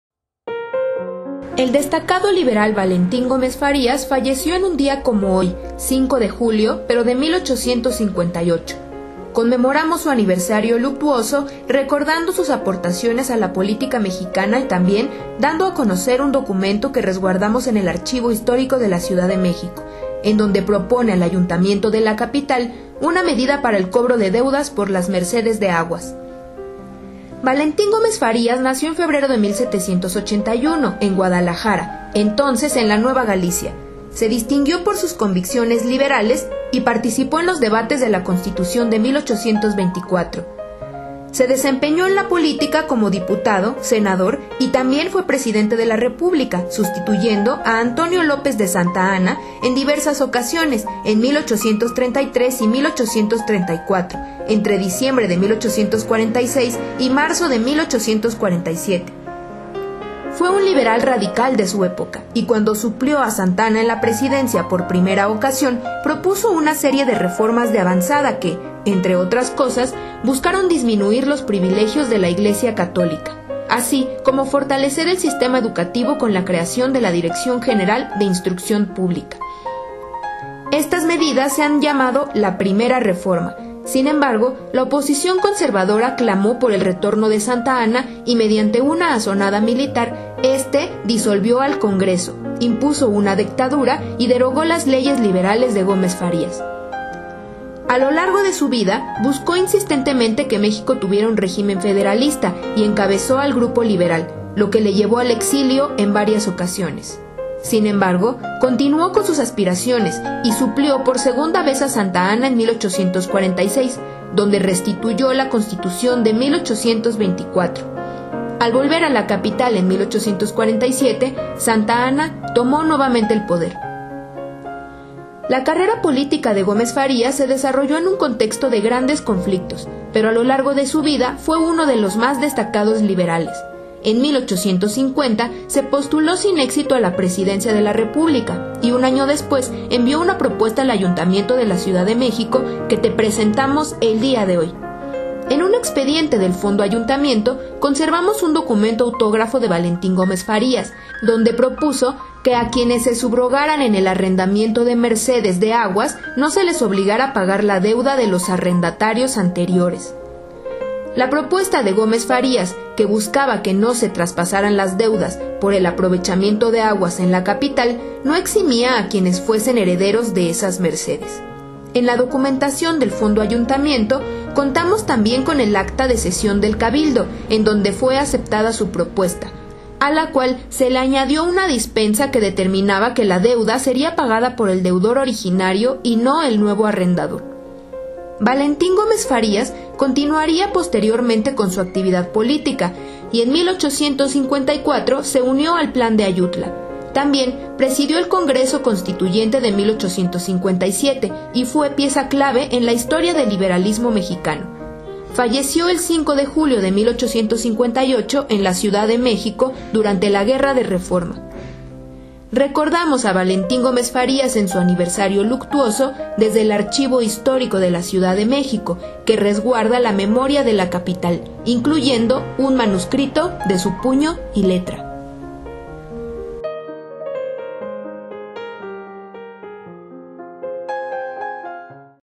Aniversario luctuoso de Valentín Gómez Farías
En esta conmemoración por el 163 aniversario del fallecimiento de Valentín Gómez Farías, te presentamos un documento de su puño y letra que resguardamos en el Fondo Ayuntamiento del Archivo Histórico de la Ciudad de México, Carlos de Sigüenza y Góngora, perteneciente a la Secretaría de Cultura Ciudad de México del Gobierno de la Ciudad de México.

¡Los documentos de este archivo son patrimonio de las y los capitalinos!